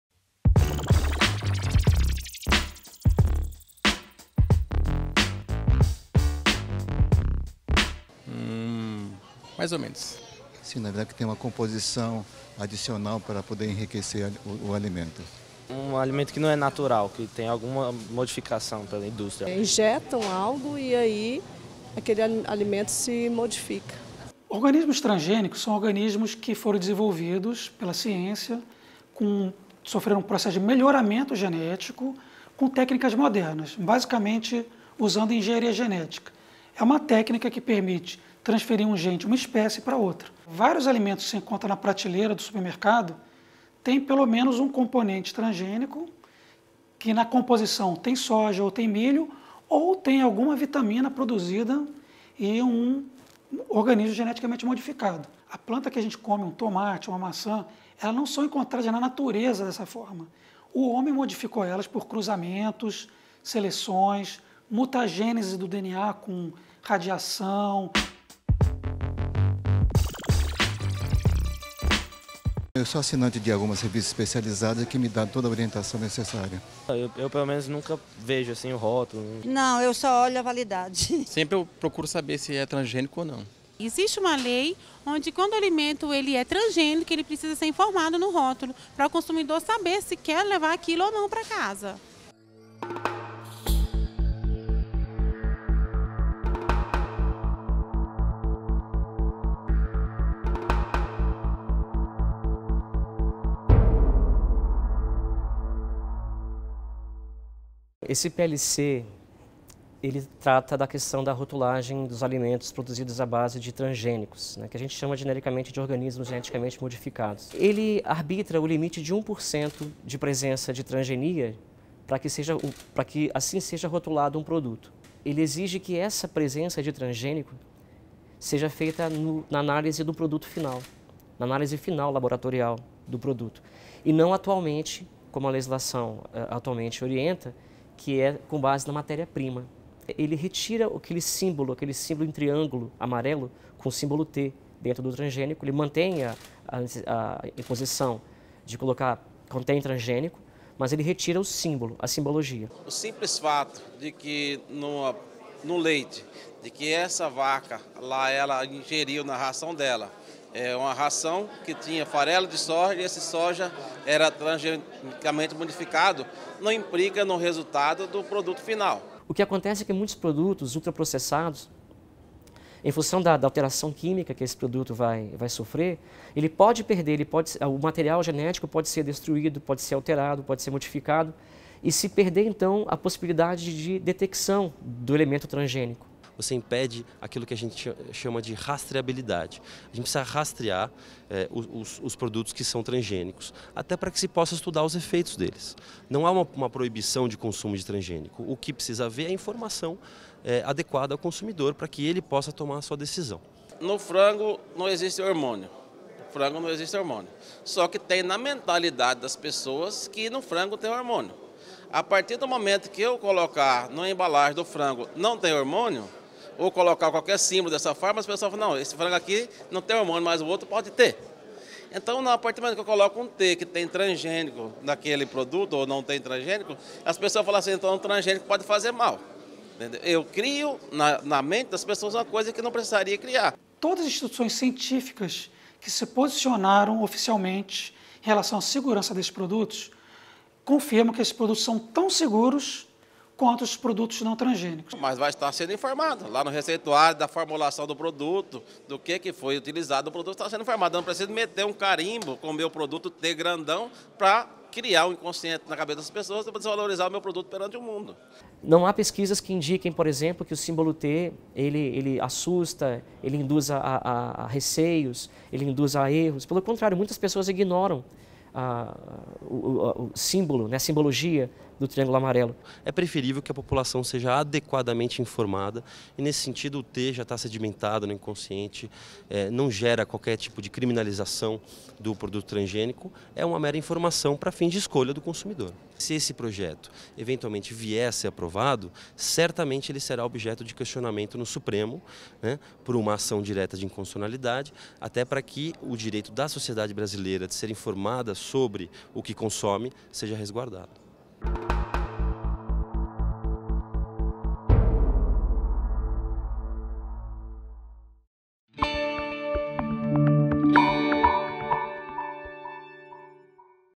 Senado analisa projeto que muda regras de identificação e rotulagem de produtos transgênicos
Projeto de Lei da Câmara 34, de 2015, propõe mudanças no processo de identificação e rotulagem de produtos transgênicos e/ou derivados de Organismos Geneticamente Modificados - OGM.